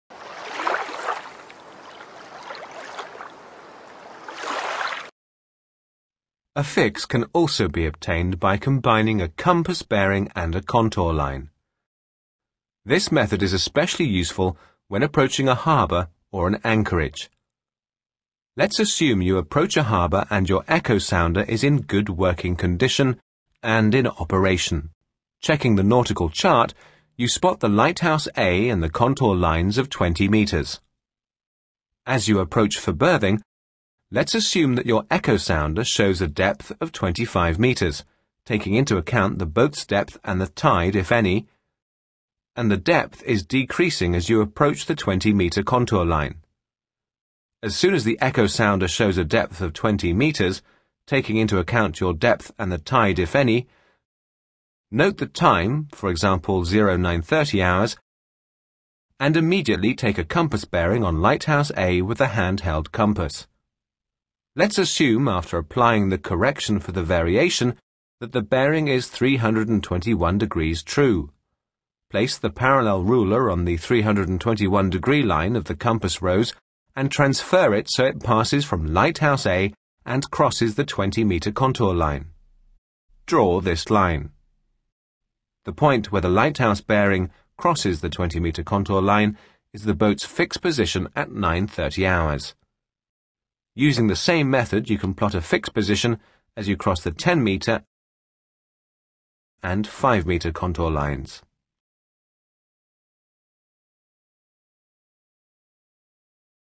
Fix position compass bearing and visual range compass bearing and visual range
A Fix can also be obtained by combining a compass bearing and a visual range.
Assume your course is 360° degrees, or North, and your speed is 7 knots.
While looking at the nautical chart you spot the lighthouse A and the range marks B and C.
While waiting for the two range marks B and C to come visually in line, go to the nautical chart and connect them with a line, draw the first LOP, crossing the course line.
Observe the two range marks, and as soon as they are  in line, write down the time, let's say 0930 hours, and immediately take a bearing on lighthouse A using the hand held compass.
let's assume that after applying the variation correction the lighthouse bearing is 045° degrees true bearing.
Place the parallel ruler on the compass rose at the 045° degree line and transfer that line passing from lighthouse A and crossing the course line.
As you can now see, the lighthouse bearing crosses the pre-drawn LOP of the range marks.
This point of crossing represents your fix position at 0930 hours.